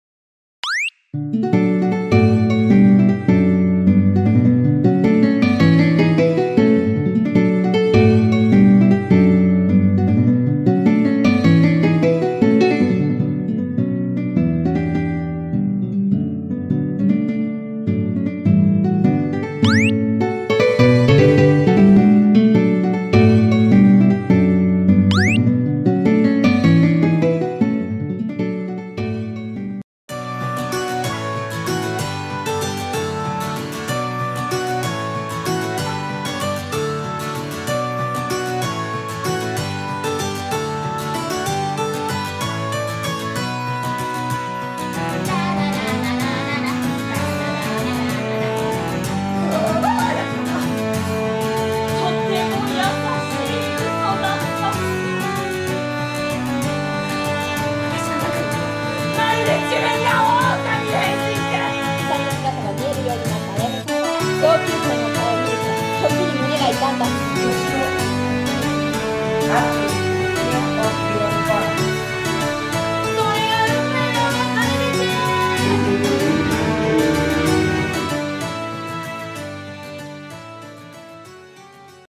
エジンバラ演劇祭で三島由紀夫と「附子」を上演したい！！
国際的な演劇祭であるエジンバラ演劇祭に日本からの参加者があまりにも少ないことを憂えた主催者が有志の女子６人でユニットを結成！
三島由紀夫の「綾の鼓」と狂言の「附子」をアレンジした作品を同演劇祭で上演する。